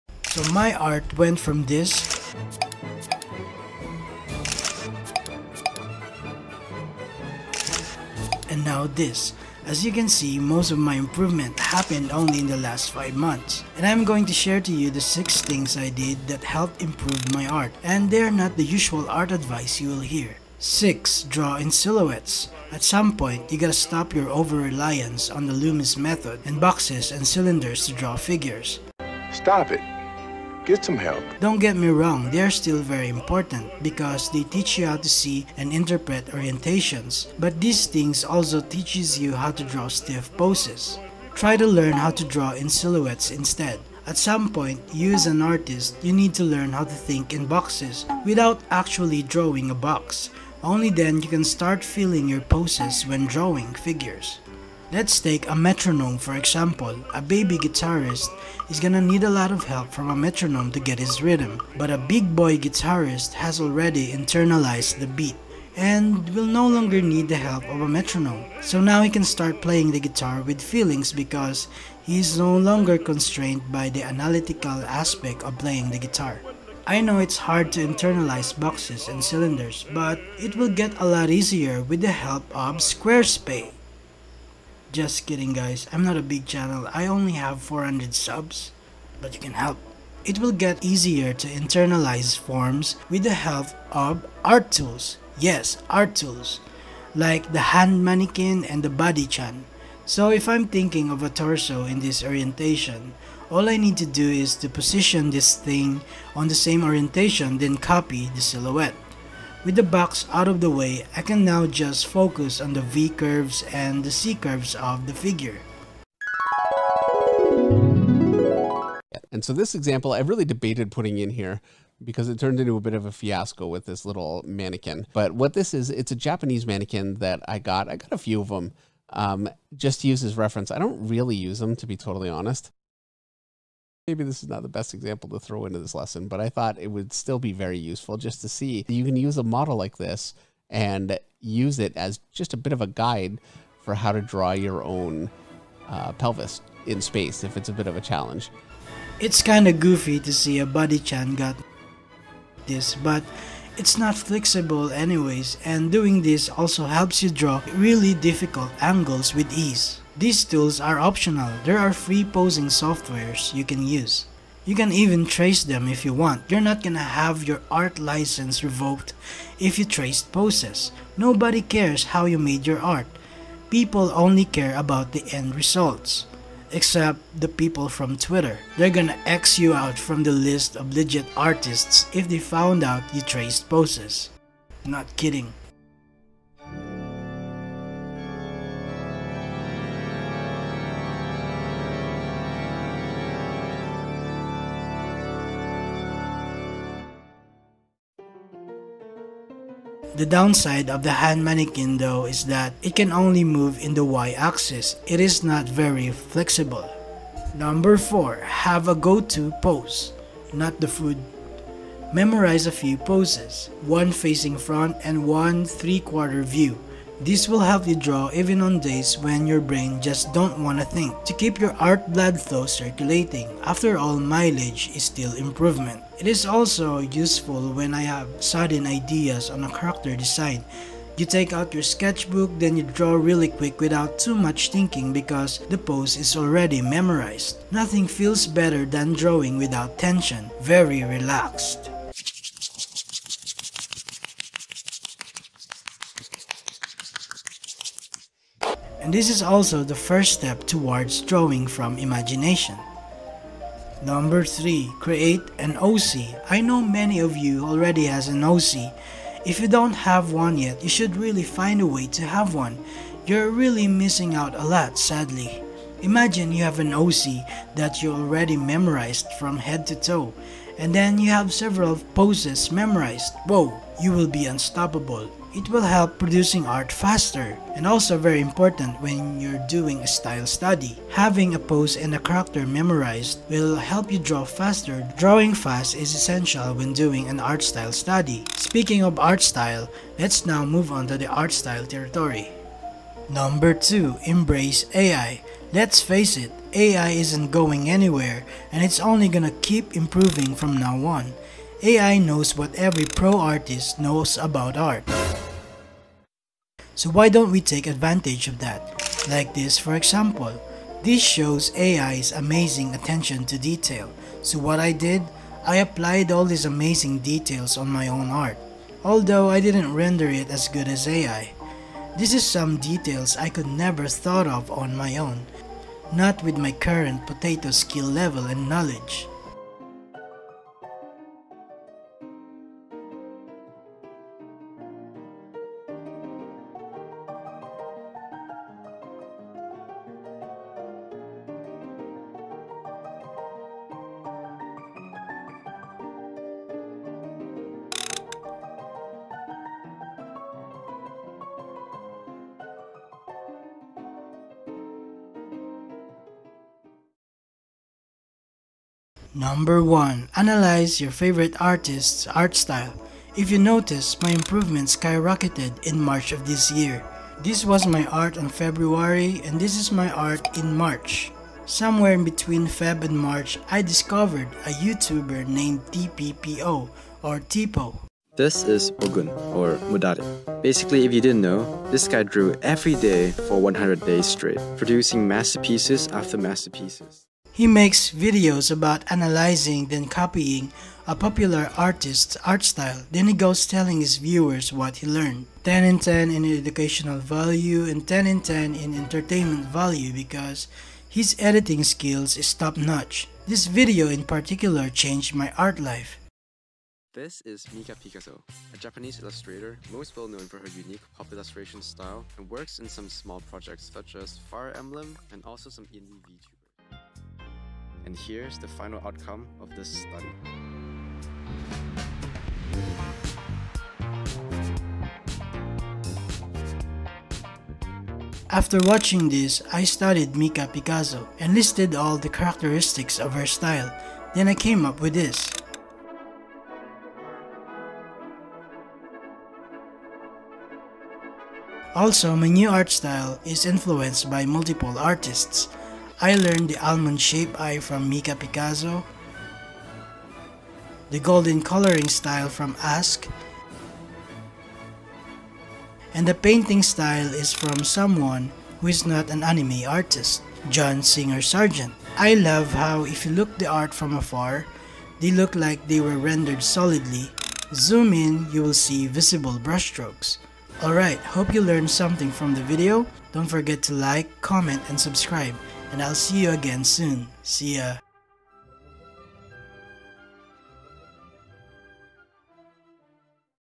6 Tips to SKYROCKET IMPROVEMENT in ART (2023)
In this video, I'm going to share to you the 6 things I did that helped improve my art in just 1 month.